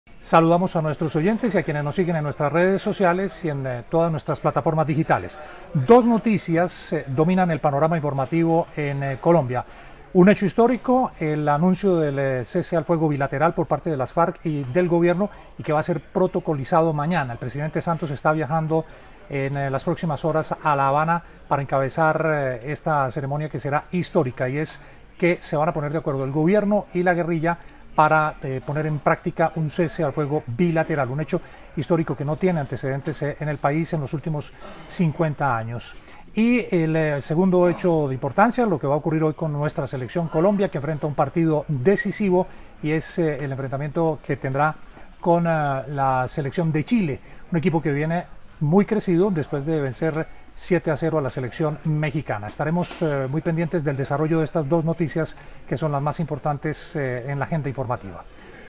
Agenda informativa - William Calderón junio 22
‪#‎ExclusivoDigital‬ Conozca los hechos que fueron y serán noticia en las próximas horas con el informe de William Calderón.

Encuentre la información en detalle ingresando a Blu Radio Colombia, canal de YouTube